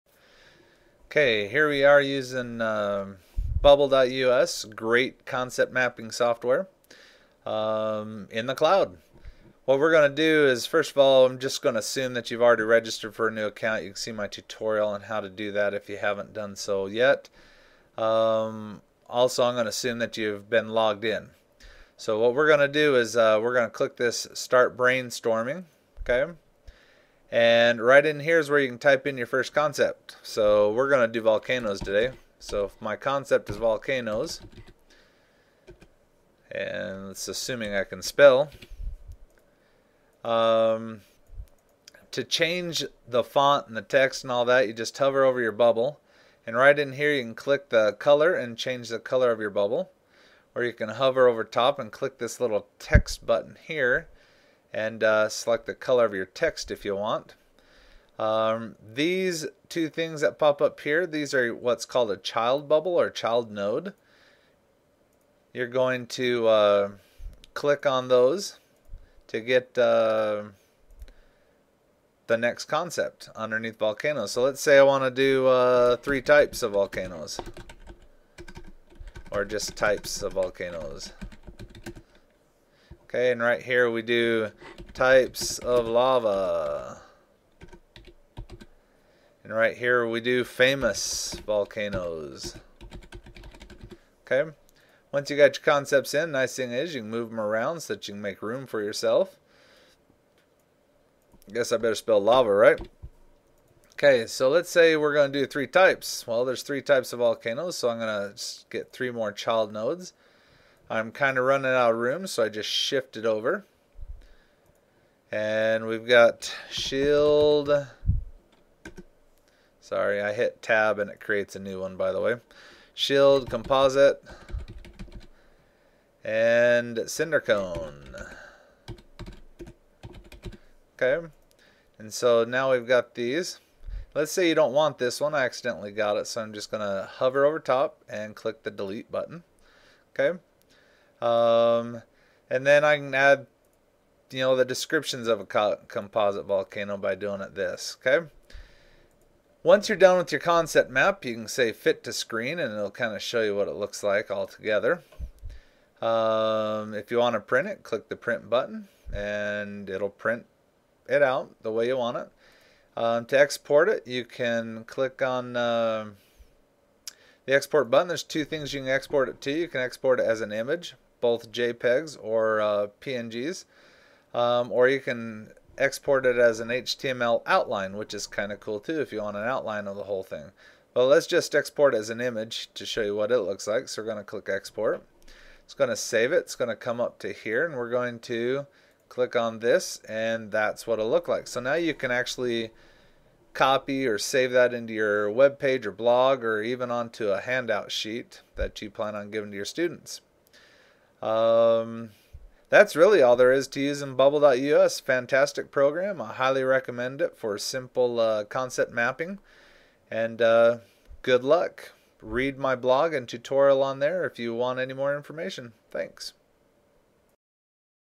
Concept mapping tutorial using Bubbl.us
You can create a simple concept map using bubbl.us. Easy to use and free. This video is a tutorial on its use.